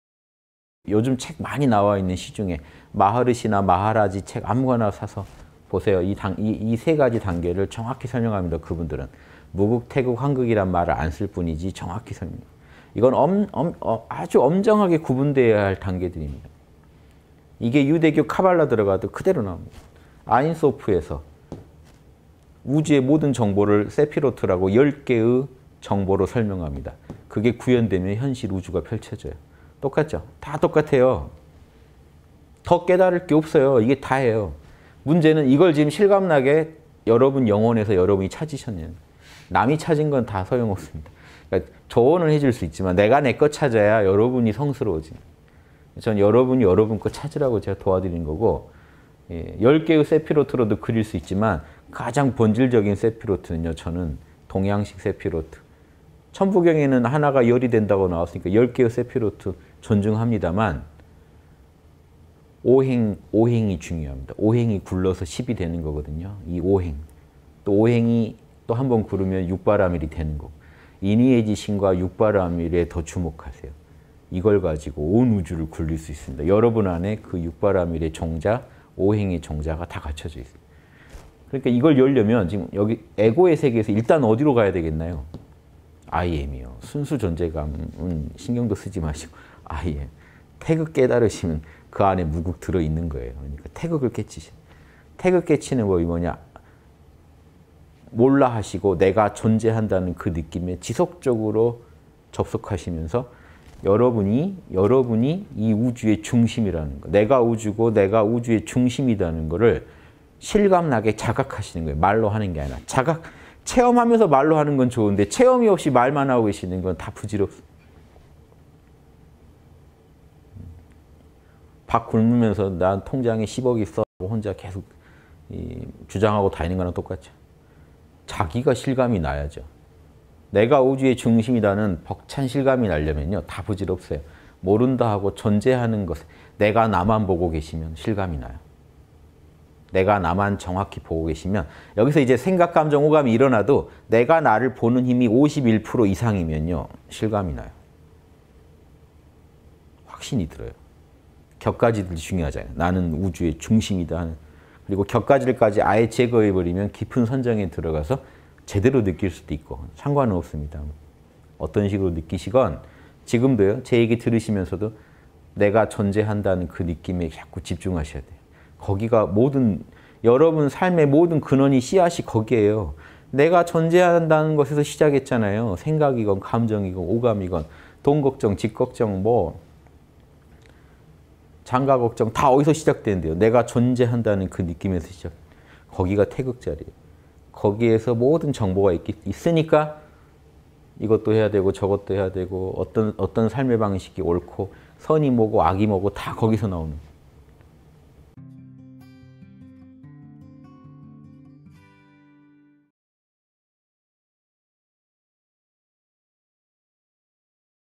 [3분 인문학] 태극 깨치는 법 _홍익학당.윤홍식
(170809 수밤 대담) 요즘 책 많이 나와 있는데 시중에,
마하리쉬나 마하라지 책 아무거나 사서 보세요.
이 세 가지 단계를 정확히 설명합니다, 그분들은.
무극, 태극, 황극이란 말을 안 쓸 뿐이지
정확히 설명해요.
이건 아주 엄정하게 구분되어야 할 단계들입니다.
이게 유대교 카발라 들어가도 그대로 나옵니다.
아인소프(Ain Sof)에서
우주의 모든 정보를 세피로트(Sephiroth)라고
10개의 정보로 설명합니다.
그게 구현되면 현실 우주가 펼쳐져요.
똑같죠? 다 똑같아요.
더 깨달을 게 없어요. 이게 다예요.

문제는 이걸 지금 실감 나게
여러분 영혼에서 여러분이 찾으셨냐는 겁니다.
남이 찾은 건 다 소용없습니다.
조언은 해줄 수 있지만
내가 내 것 찾아야 여러분이 성스러워지는 거죠.
전 여러분이 여러분 것 찾으라고 도와드리는 거고.

10개의 세피로트로도 그릴 수 있지만
가장 본질적인 세피로트는
동양식 세피로트.
천부경에는 하나가 10이 된다고 나왔으니까,
10개의 세피로트 존중합니다만
5행이 중요합니다.
5행이 굴러서 10이 되거든요.
5행이 또 한 번 구르면 6바라밀이 되고.
인의예지신과 6바라밀에 더 주목하세요.
이걸 가지고 온 우주를 굴릴 수 있습니다.
여러분 안에 그 6바라밀의 종자,
5행의 종자가 다 갖춰져 있습니다.

이걸 열려면 에고의 세계에서
일단 어디로 가야 되겠나요?
‘I AM(아이 엠)'이요.
순수 존재감은 신경도 쓰지 마시고, I AM.
태극 깨달으시면 그 안에 무극 들어있는 거예요.
태극을 깨치셔야 해요.

태극 깨지는 게 뭐냐?
‘몰라’ 하시고 내가 존재한다는 그 느낌에
지속적으로 접속하시면서
여러분이 이 우주의 중심이라는 것,
‘내가 우주고 내가 우주의 중심이다.’
는 것을 실감나게 자각하시는 거예요.
말로 하는 게 아니라.
체험하면서 말로 하는 건 좋은데
체험이 없이 말만 하고 계시는 건 다 부질없어요.
밥 굶으면서 ‘나 통장에 10억 있어.’
혼자 계속 주장하고 다니는 것과 똑같죠.
자기가 실감이 나야죠.

‘내가 우주의 중심이다.’ 하는
벅찬 실감이 나려면 다 부질없어요.
‘모른다’ 하고 존재하는 것.
내가 나만 바라보고 계시면 실감이 나요.
내가 나만 정확히 보고 계시면
여기서 생각, 감정, 오감이 일어나도
내가 나를 보는 힘이 51% 이상이면 실감이 나요.
확신이 들어요.
곁가지들 중요하지 않아요.
난 우주의 중심이다는.
그리고 곁가지들까지 아예 제거해 버리면
깊은 선정에 들어가서 제대로 느낄 수도 있고
상관은 없습니다.

어떤 식으로 느끼시건 지금도 제 얘기 들으시면서도
내가 존재한다는 그 느낌에 자꾸 집중하셔야 해요.
거기가 여러분 삶의 모든 근원의 씨앗이 거기예요.
내가 존재한다는 것에서 시작했잖아요.
생각이건, 감정이건, 오감이건,
돈 걱정, 집 걱정, 뭐 장가 걱정
다 어디서 시작되는데요?
내가 존재한다는 그 느낌에서 시작되죠.
거기가 태극 자리예요.
거기에 모든 정보가 있으니까
이것도 해야 하고, 저것도 해야 하고,
어떤 삶의 방식이 옳고, 선이 뭐고 악이 뭐고,
다 거기서 나옵니다.